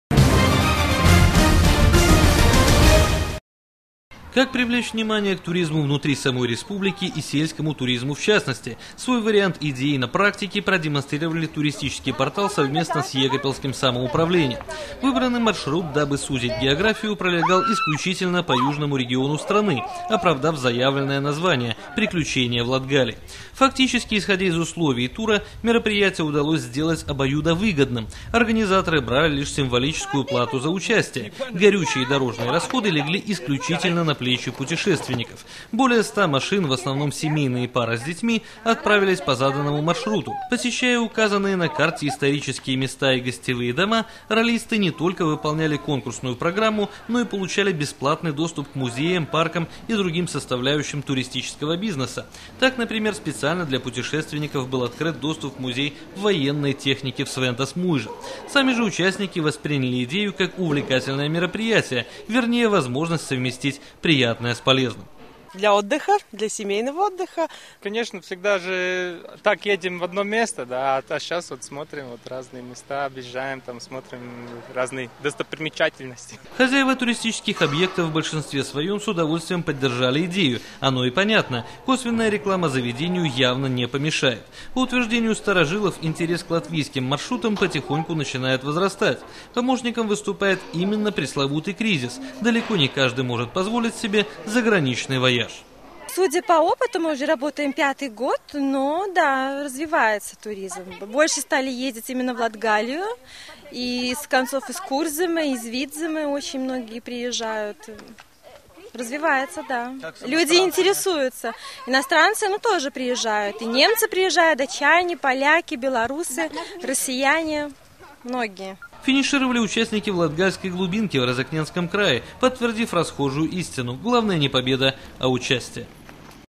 Туристическое ралли: Латгалия
С любопытной инициативой выступил латвийский портал, который организовал народное ралли по туристическим маршрутам Латгалии. Как это выглядило на практике -- в материале Владимира Христова.